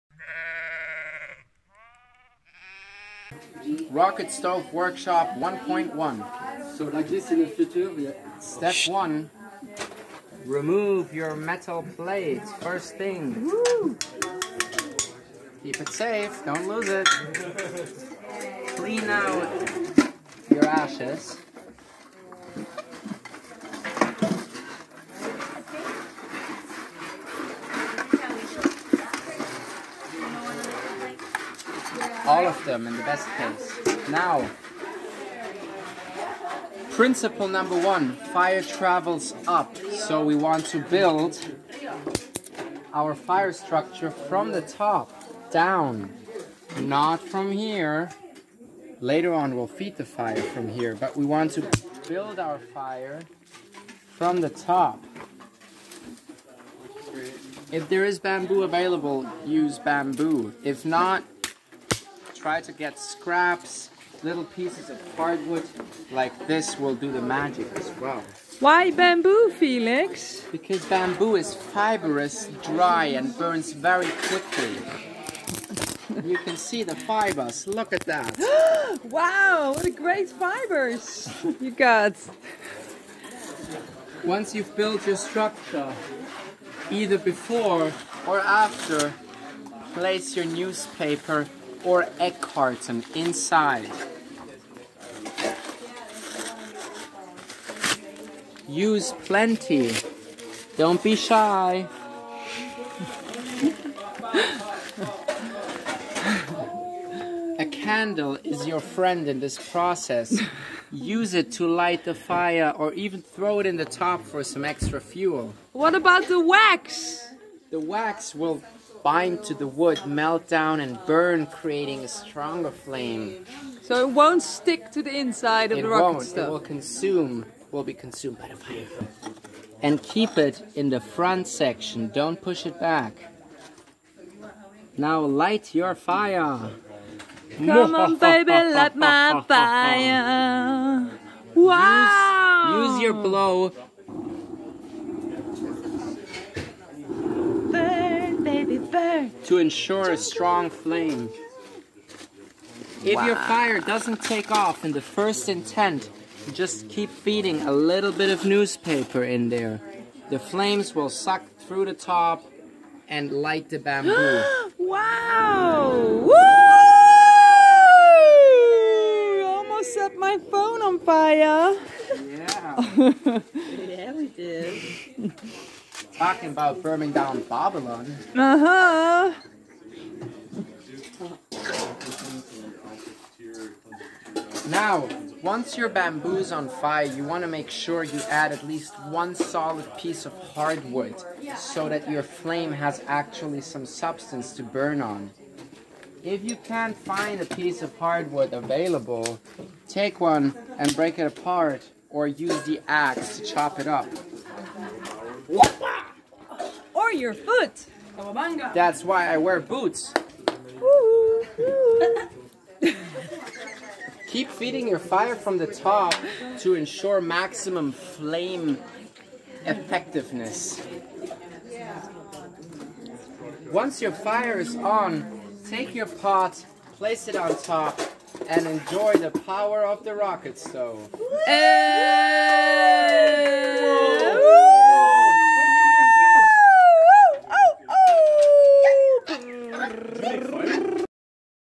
Rocket stove workshop 1.1
How to use a rocket stove at Finca Morpho taught by our snazzy kitchen pillar whiz Felix the Phoenix

produced by Sean Ray Ariel Canning